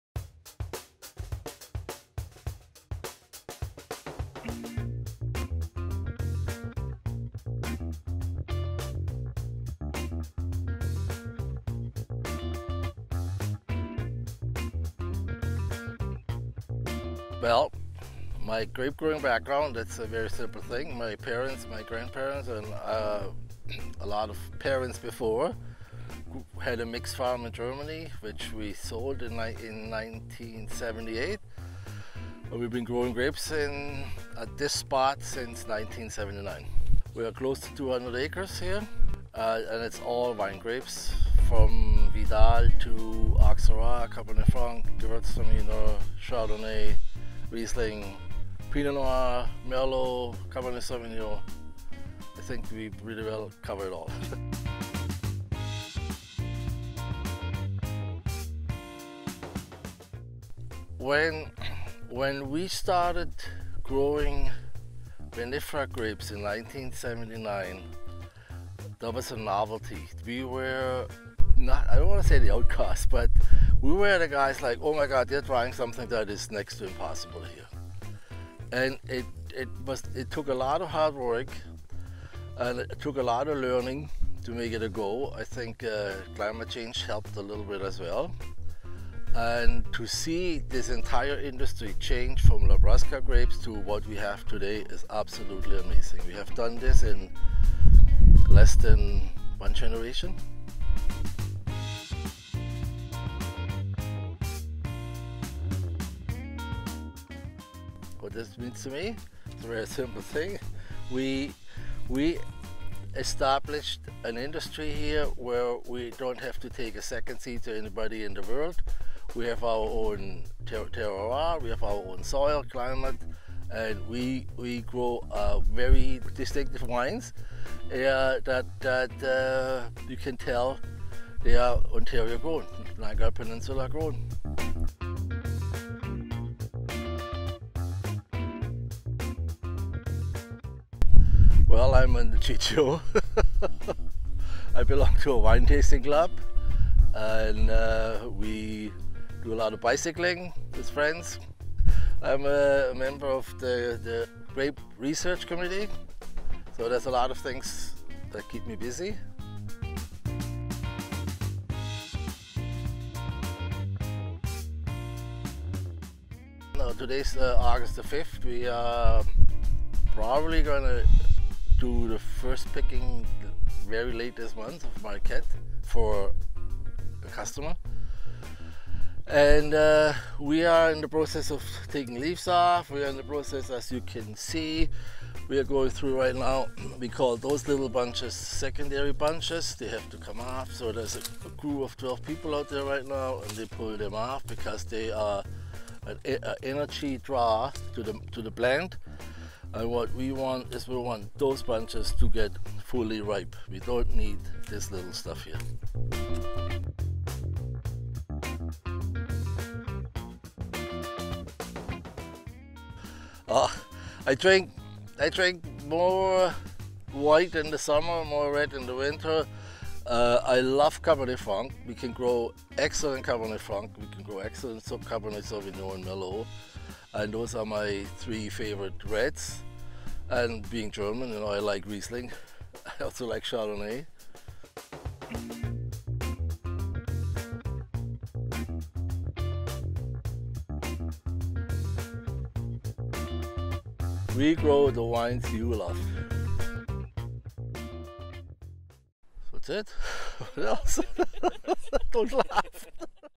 BetweenTheVines - Episode 3: Farm Families Make a Difference
In this episode, we headed over to Seeger Vineyards to talk with Albrecht Seeger to learn a little bit about the importance and history of Grape Growers and farm families.

If you liked this video, be sure to let us know, and don't forget to check out for more information on the Ontario Grape and Wine industry!